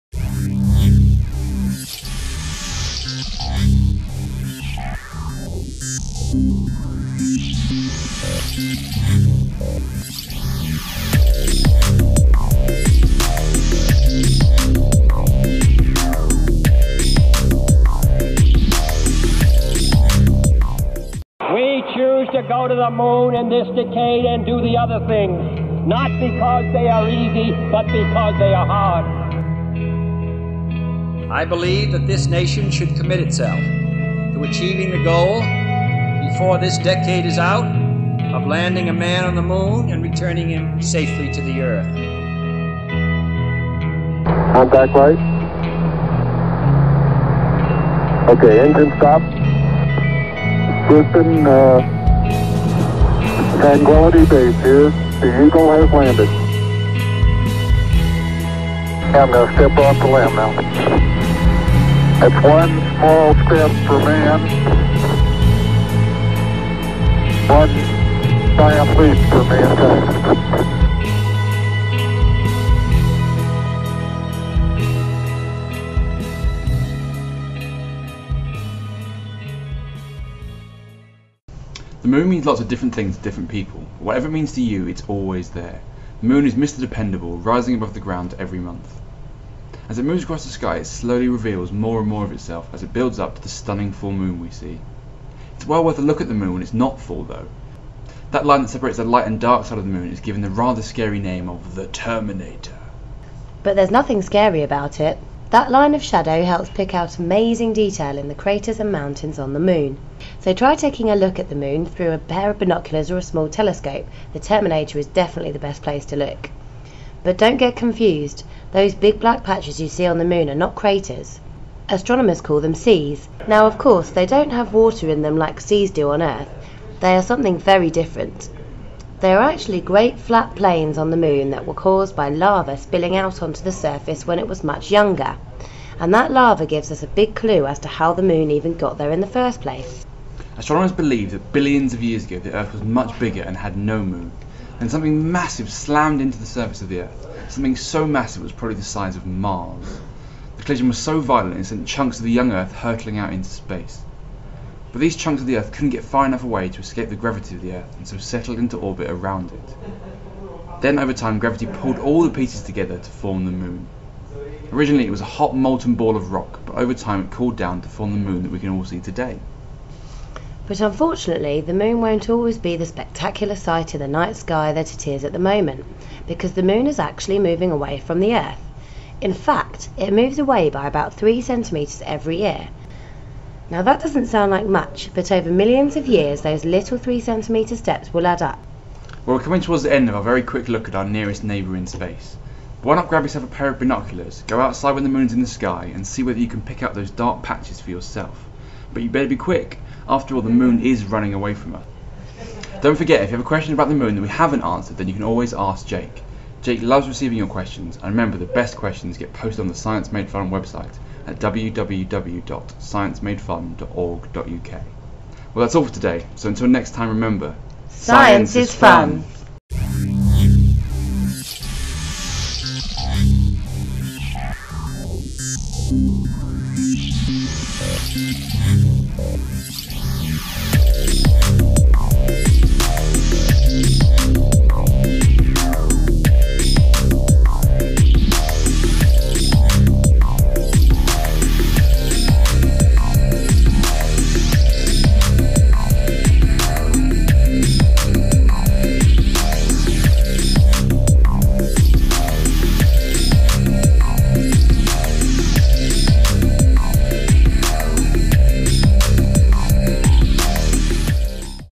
The Moon
Join Colin and Catherine as the explore the beautiful sight that is the Moon. You'll find tips about the best ways to look at the Moon for yourself as well as finding out how the Moon was made

Follow Science Made Fun founder Colin on Twitter for more science goodness!